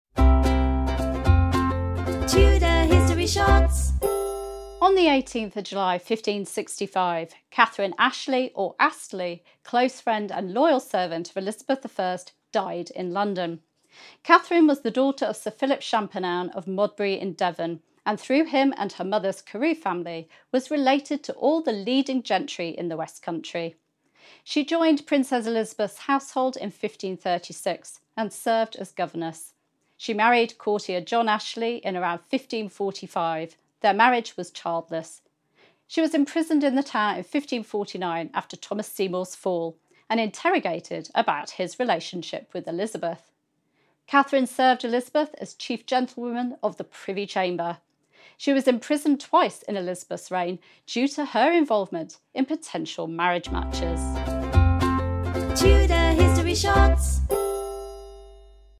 18 July - Kat Ashley, Elizabeth I's lady
On this day in Tudor history, 18th July 1565, in the reign of Queen Elizabeth I, the queen's close friend and loyal servant, Kat Ashley, died.

Find out more about Kat (also known as Katherine Ashley, Katherine Astley and Katherine Champernowne) in this edition of TudorHistoryShorts from Claire Ridgway.